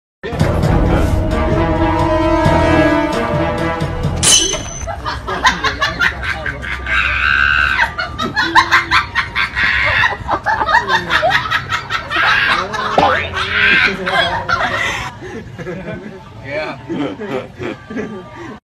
CÁCH XỬ LÝ XE MÁY ĐỖ TRÁI QUY ĐỊNH (VÀ KHÓA CỔ XE) BẰNG XE ĐẠP
Nguồn: Gr Không Sợ Chó | Spyer, Đinh Công Lượng | Tik Tok ID: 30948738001